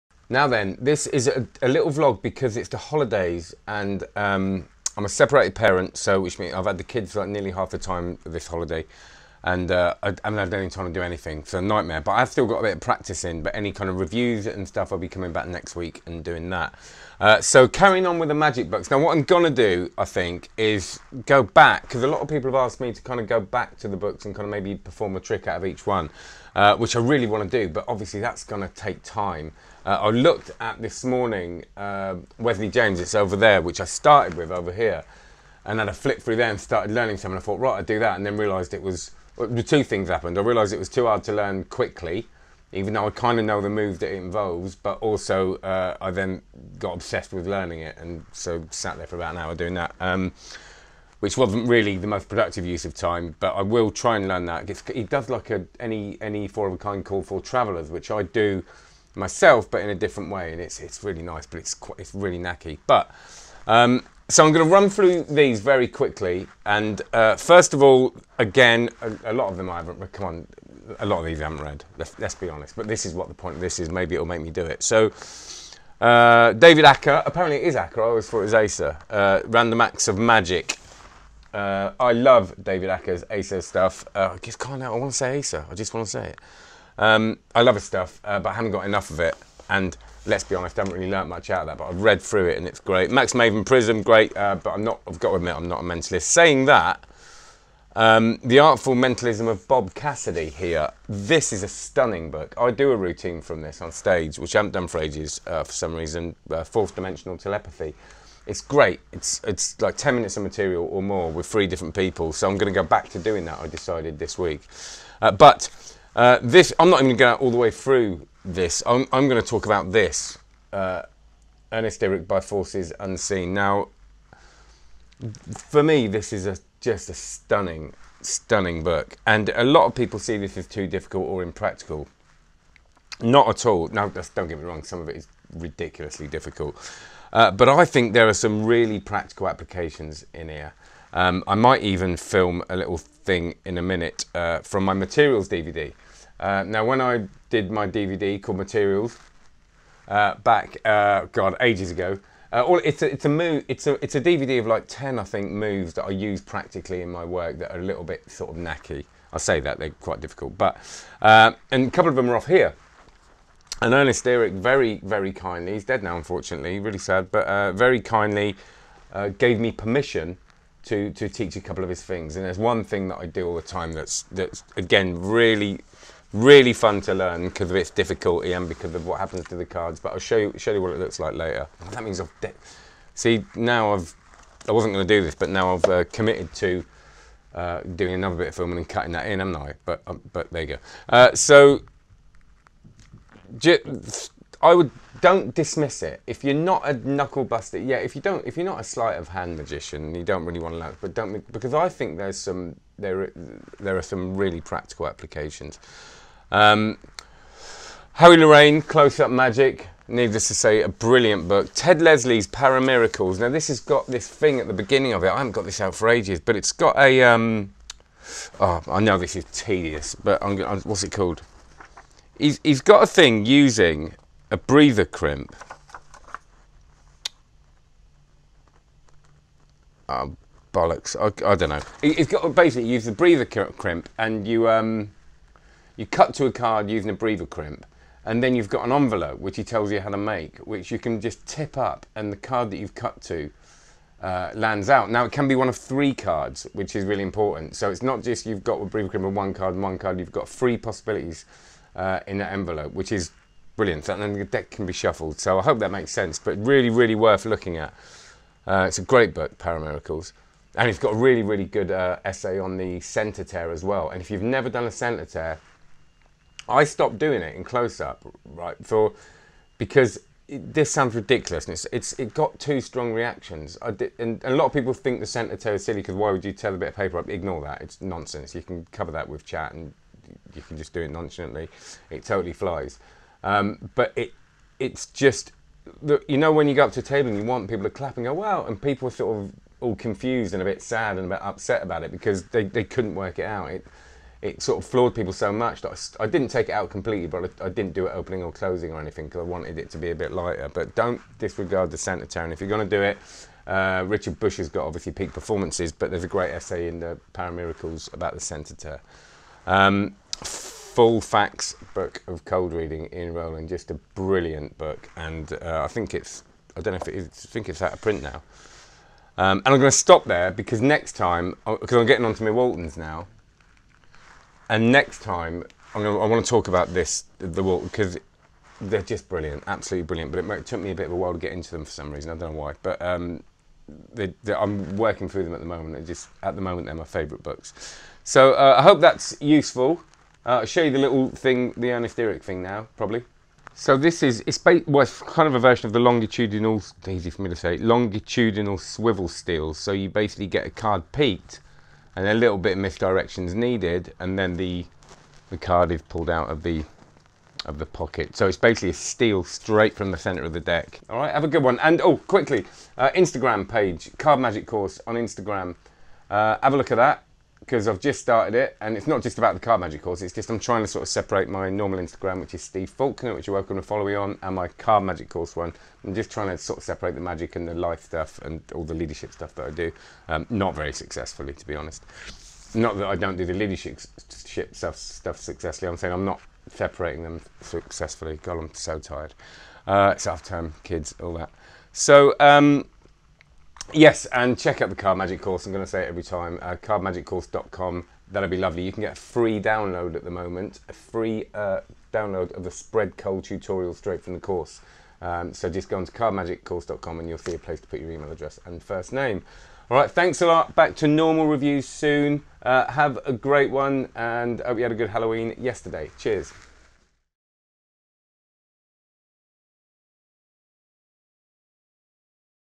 Real Magic Vlog #3: Cassidy, Lesley and Earick
Another ramble through my book collection, I'll soon be going back and looking into more detail. Any suggestions, fire away!